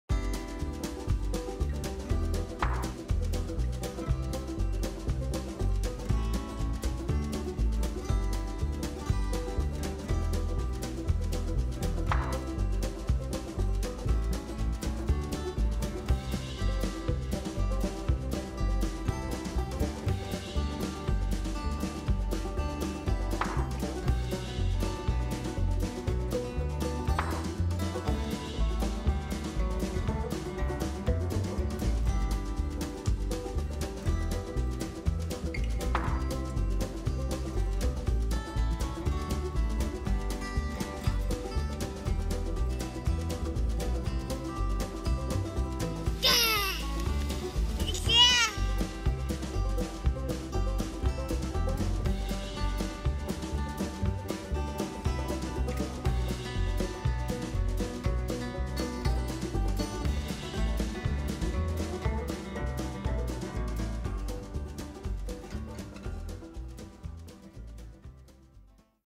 APRENDA A FAZER UM BONECO DE NEVE (em 1 min) | Vida no Canadá | Fer & Van
PRIMEIRA VEZ MONTANDO UM BONECO DE NEVE!

Se você quer montar ou tem curiosidade para saber como se monta um boneco de neve, esse vídeo é pra você. Nós mostramos pra você, em 5 passos como fazer para montar um boneco de neve.

Sonoplastia: Marcello Óg